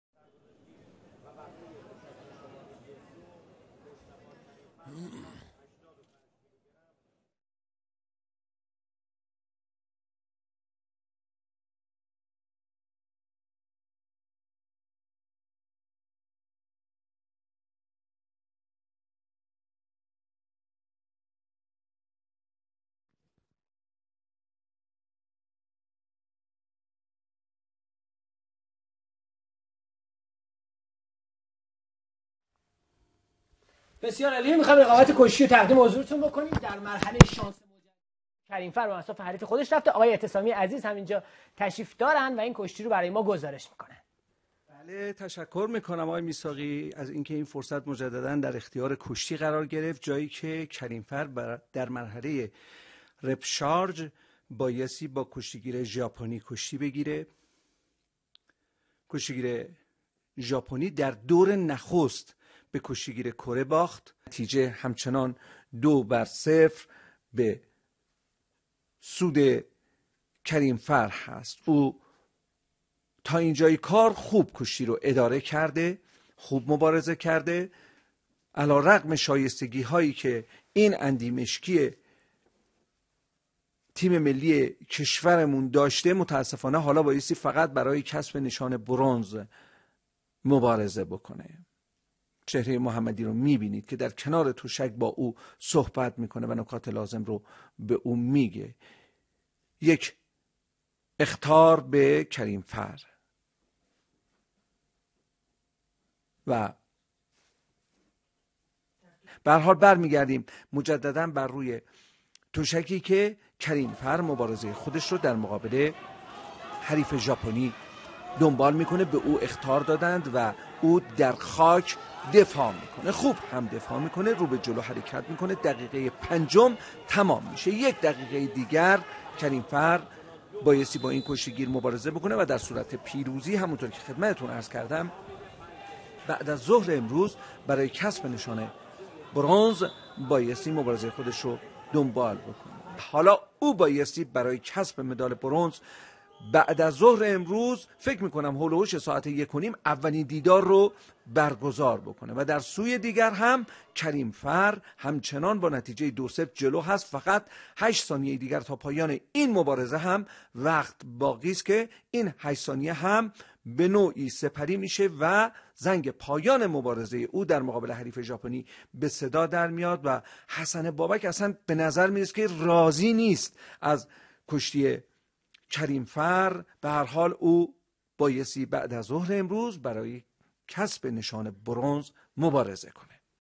karimfar japan- asian games 2014 wrestling- کریم فر
پیروزی کریم فر مقابل نماینده japan

کشتی فرنگی-بازی های آسیایی اینچئون ۲۰۱۴